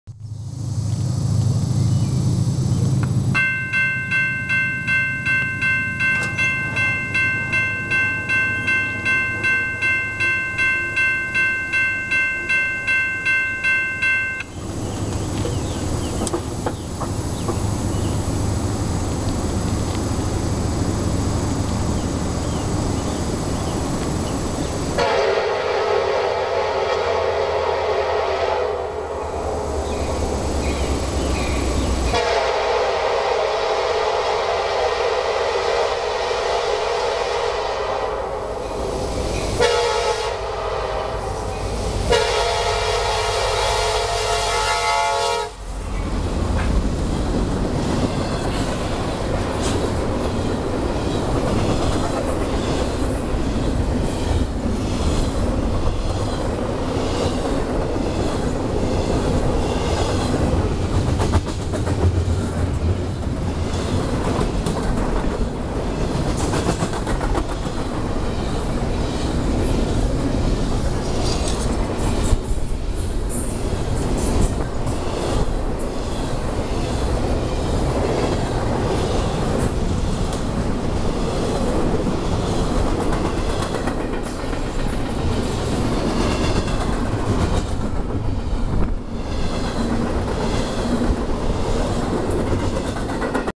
CSX Q123 at Lyons, NY 7/14/12
CSX train Q123 is seen at the Sunderville Road crossing in Lyons, NY on 7/14/12.

CSXT 4790
CSXT 5372
CSXT 5274

Track 1W/ 162/ 47mph